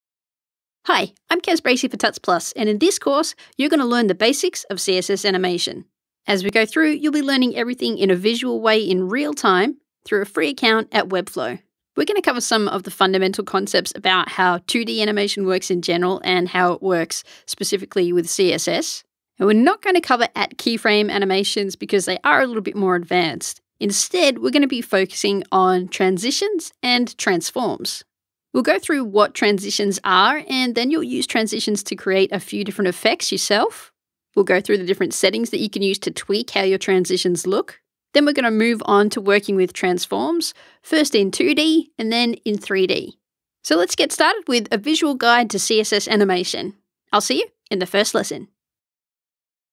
A Visual Guide to CSS Animation: Introduction
It’s now possible to create awesome animation effects using pure CSS, without a scrap of JavaScript in sight. In this course you’ll learn how to create CSS animations powered by the transition and transform properties.

And because animations are highly visual, rather than getting bogged down in code we’re going to be learning visually. We’ll be using Webflow’s popular visual web design tool (via a free account) so you’ll see your animations play out in real time, as you create them.

Note, we won't be covering @keyframe animation in this course. Instead, we'll be focusing on animation fundamentals as well as transitions and transforms.